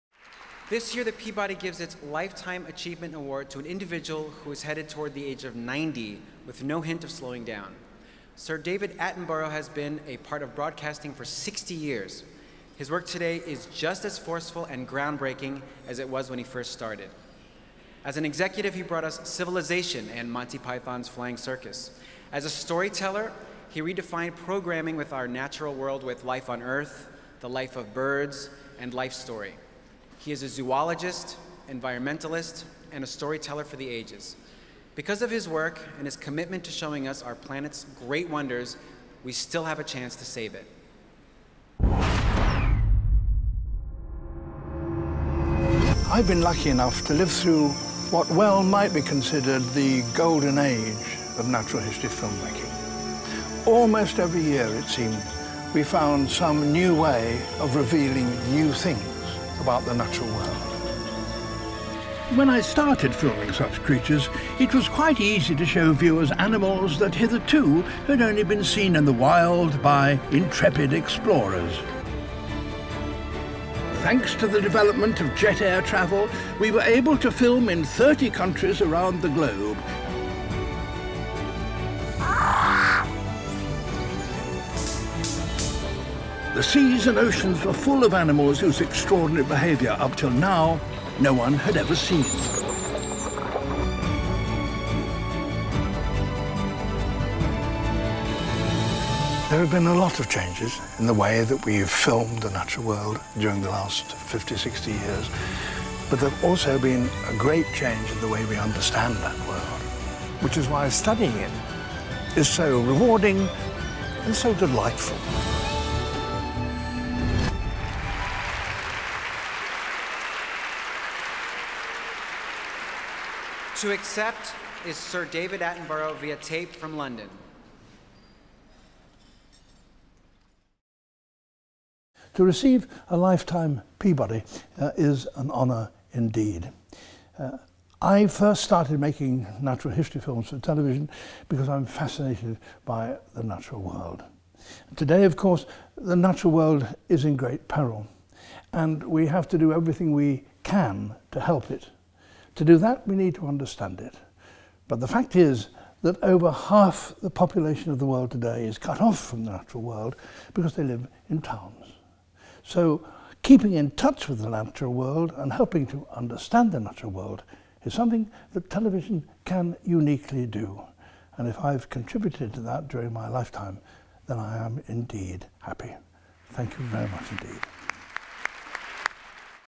Sir David Attenborough - 2014 Peabody Award Acceptance Speech
Winner 2014

On the face of it, a personal award to an 89-year-old with a broadcasting career of more than 60 years behind him would seem valedictory, but one of the many remarkable things about David Attenborough is the energy with which he is still producing cutting-edge work in the field he has dominated for the duration of that career: the television natural history program. For a time, in the 1960s and early 1970s, it seemed as though he had made the logical career progression from innovative program-maker to successful broadcasting executive. As controller of BBC2 and director of BBC Television Programmes, he gave us classics as varied as Civilisation and Monty Python’s Flying Circus. Surely heading for the top job in British broadcasting, he decided instead to get back out into the world and give it the definitive filmed account of the evolution of species: the seminal 1979 series Life on Earth. Since then, the wildlife classics have followed at regular intervals, including the Peabody-winning The Private Life of Plants (1995) and The Life of Birds (1999), each one presenting the latest advances in zoological research, using the newest recording technologies to breath-taking effect. Life Story, the first natural history series shot in ultra-high definition, would have been unthinkable without his contribution. A Personal Peabody Award goes to Sir David Attenborough, zoologist, environmentalist and communicator, for continuing to bring his wonder at the natural world into our homes.